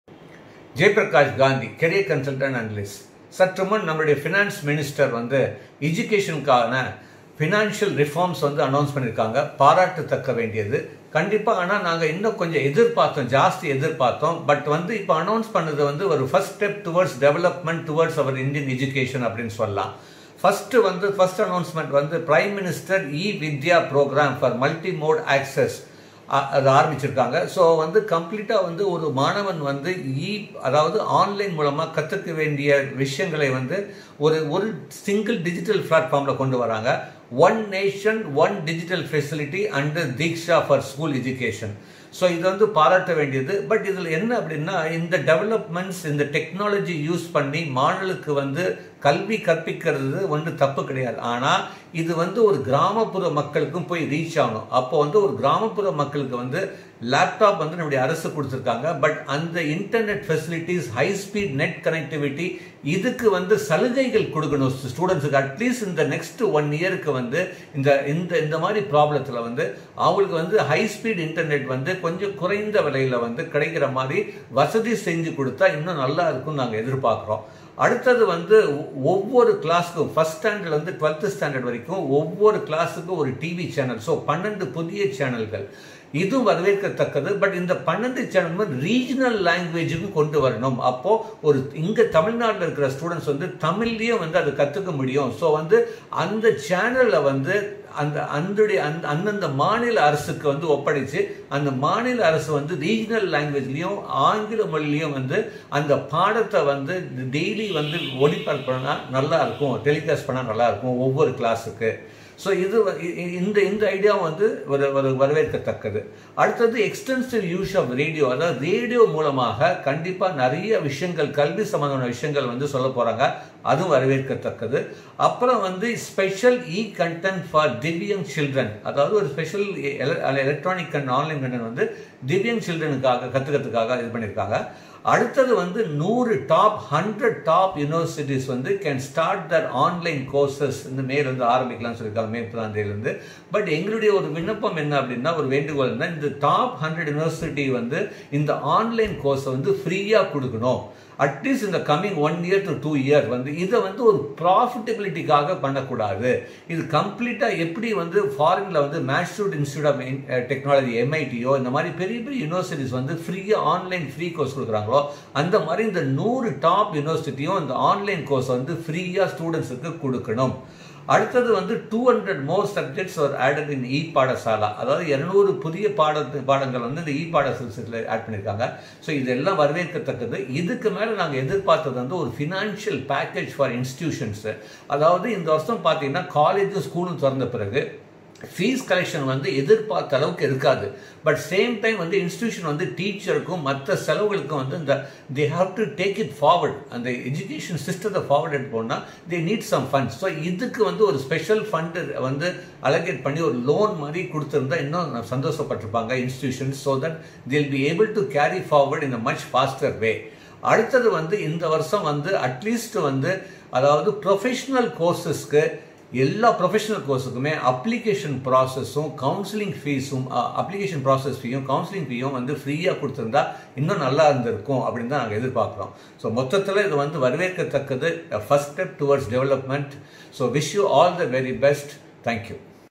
Latest Education Reforms as Announced by our FM | Insights | Suggestions | Impacts
As announced  by our honorable Finance Minister Nirmala Sitharaman, watch Career Consultant and Analyst Jayaprakash Gandhi talk about his insights, suggestions and impact towards the latest education reforms and policies like MNREGA, PM e-VIDYA etc.

By Jayaprakash Gandhi - Career Guidance and analyst for over 20 years and counseled over 2 million students across the world.